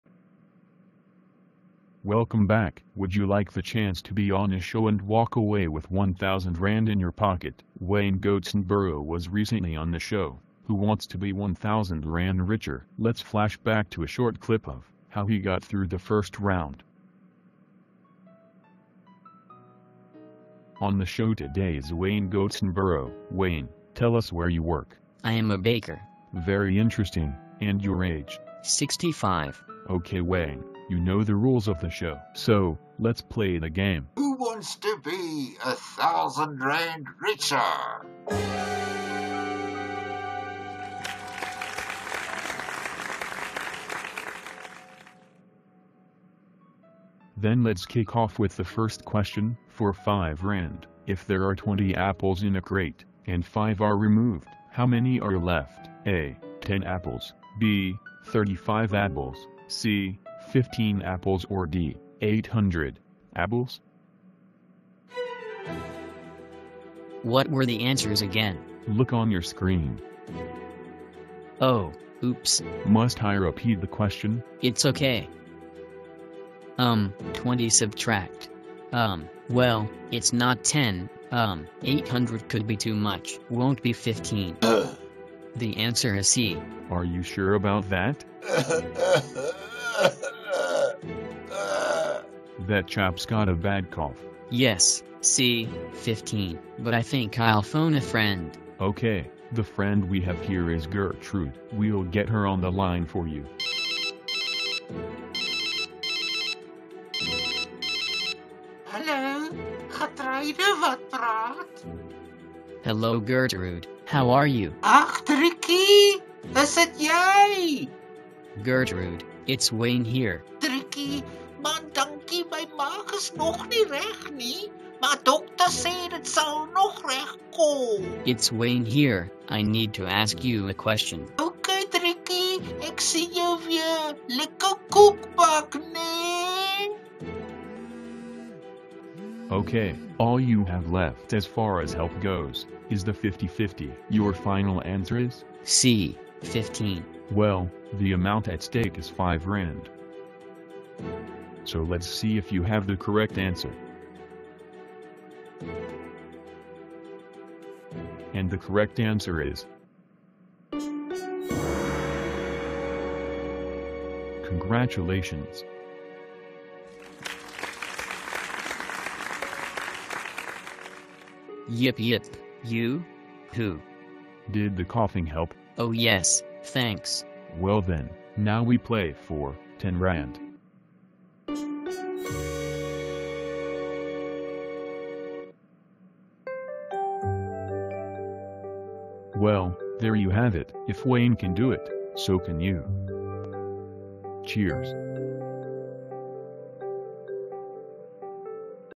Win R1,000 today, only 18yrs+ may enter
Who wants to win R1,000, quick and easy
Must be 18yrs or older with gree ID book
Enter now to win
Nothing harmful meant, purely for the fun and maybe a giggle